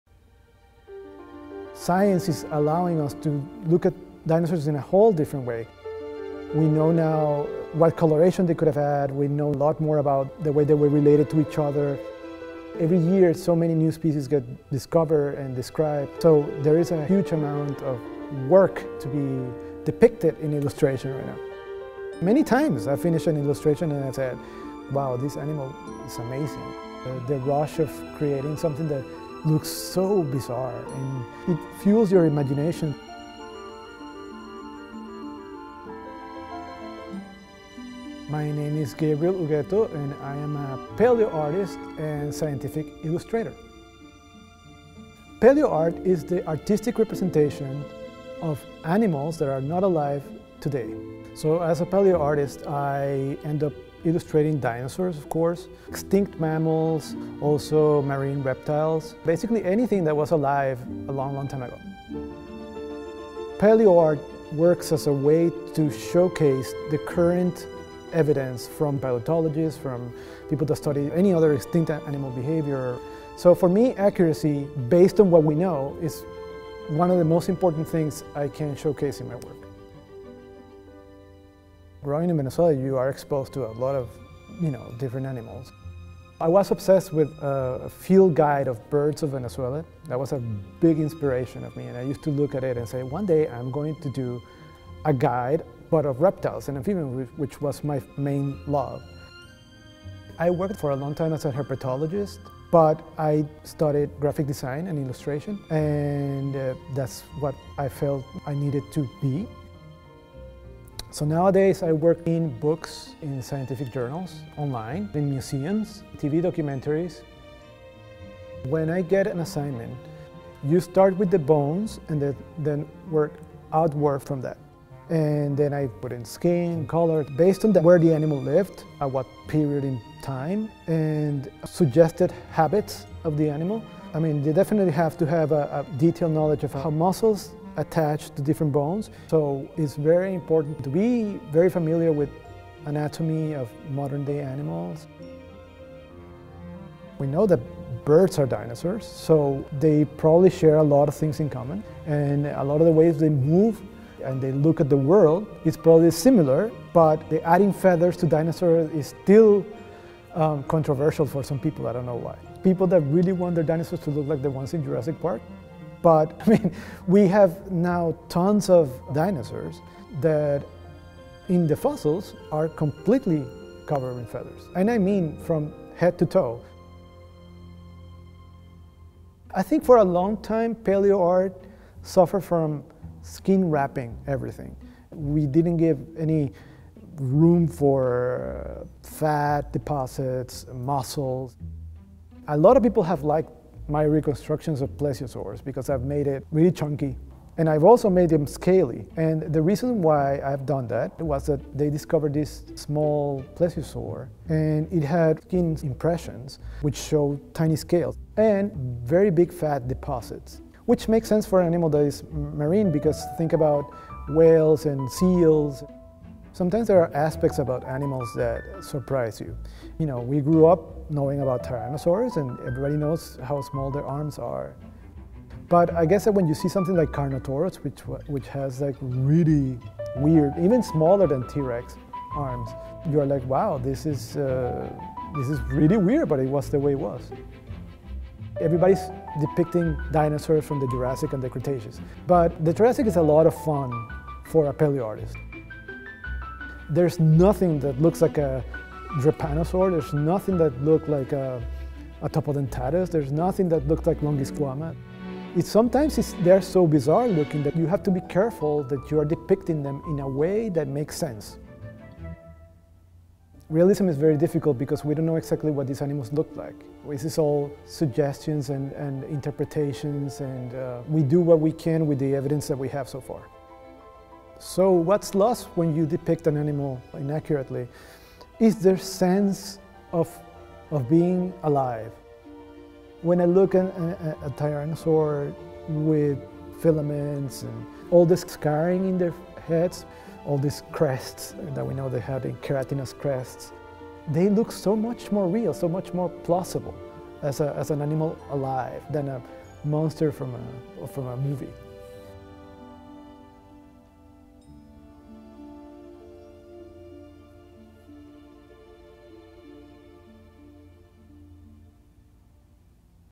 PaleoArt - New Visions of Ancient Creatures
Paleoartist and scientific illustrator Gabriel Ugueto has a golden rule for his work: Accuracy. In order to resurrect the dinosaurs, Ugueto begins with a single bone and works his way from inside out. He researches whether there are any related animals alive today, or existing fossils that may shed light on how the bone fragment fits into a larger piece, and reconstructs the entire skeletal system. He then sketches in muscle groups, and adds skin and color considering where the animal lived and during what period of time.

But his resulting illustrations often don’t match the Jurassic Park-inspired dinosaurs that we’re used to.

Illustrations by Gabriel Ugueto
Additional Images by Shutterstock and E. Frey